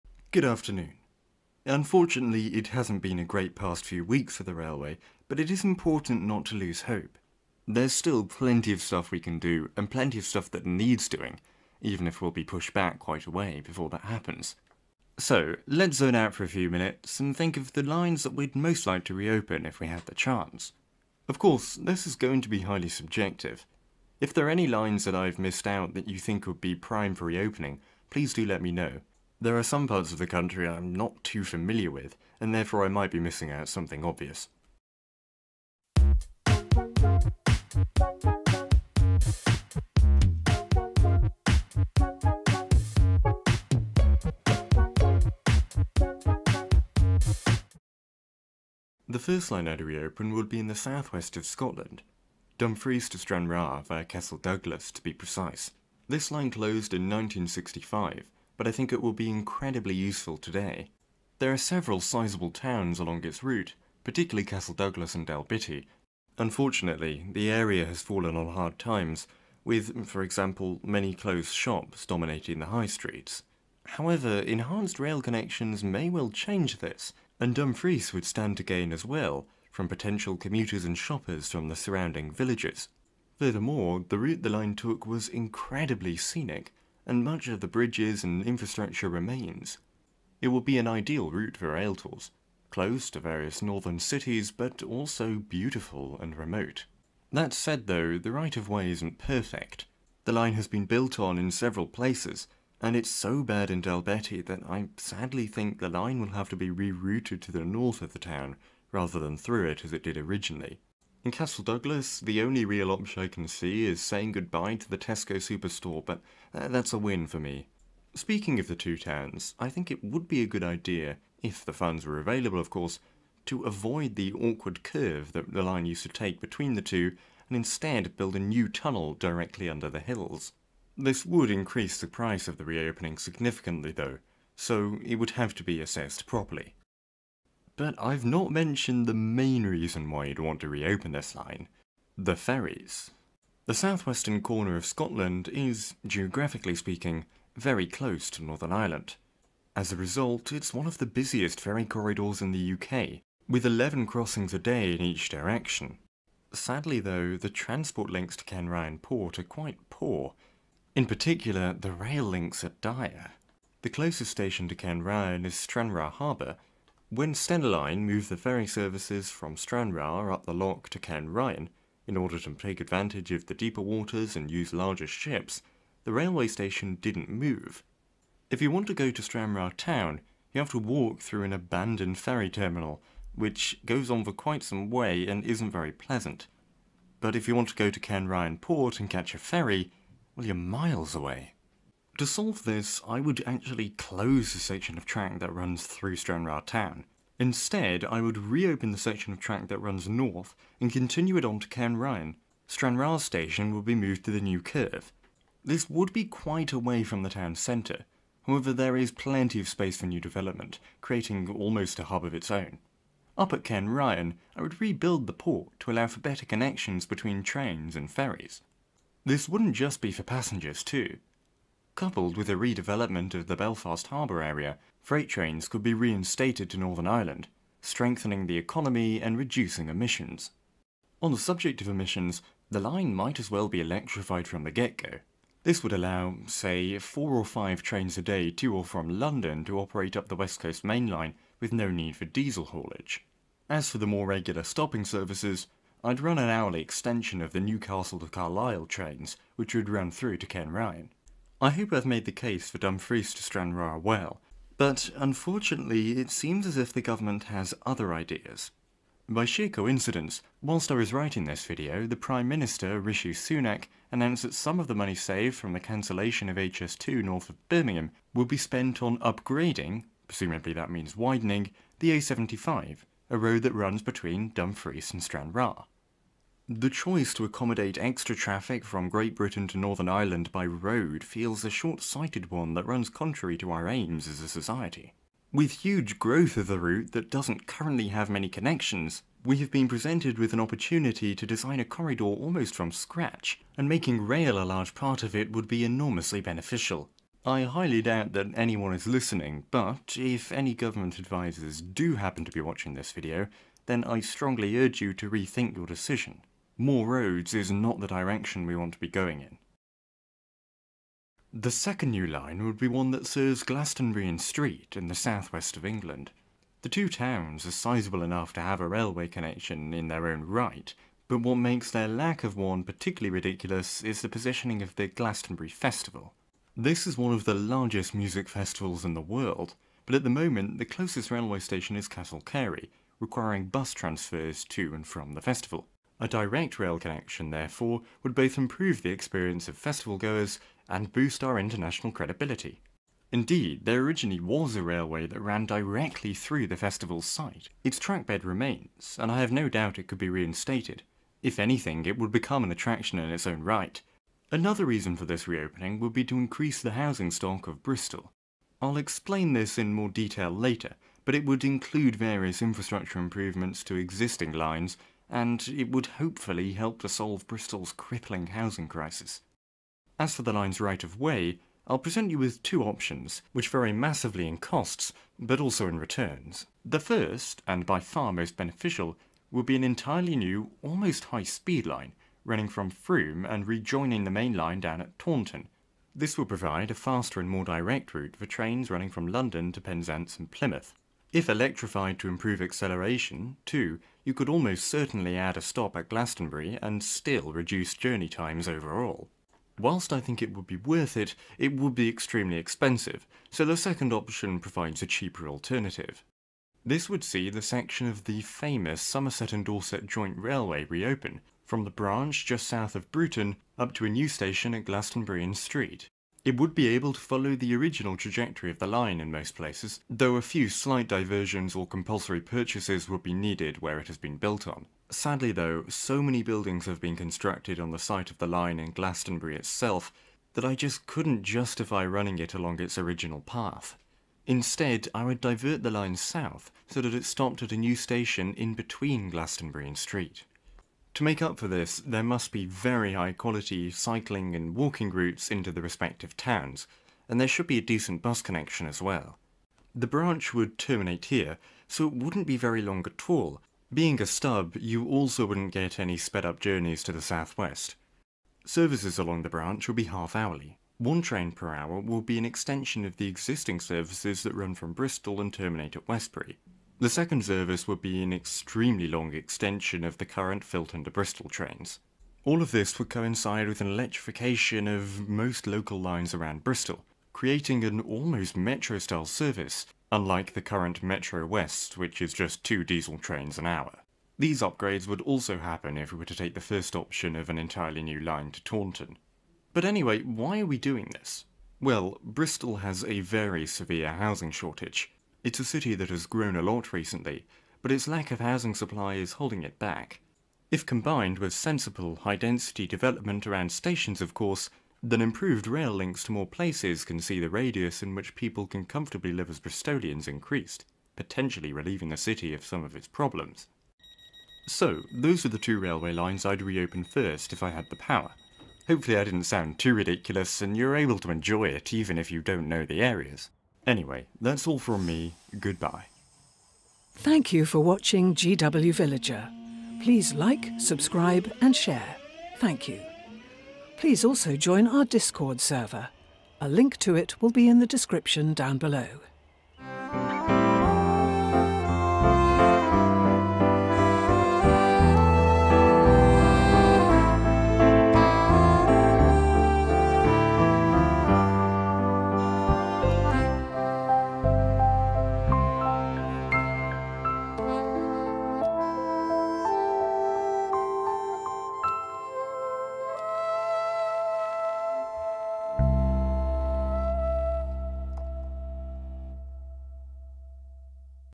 Re-opening Britain's Railways (Part 3): Dumfries - Stranraer and Glastonbury
Sadly, various recent announcements have left Britain's rail network in a worse state than it was before. But, if things were to be better, (which they will be one day) what lines would make the most strategic sense to re-open?

Information:
DfT - SPAS0201

Photography:
CNES / Airbus, Data SIO, NOAA, U.S. Navy, NGA, GEBCO - Google Earth

GWR & Ordnance Survey

TravelWest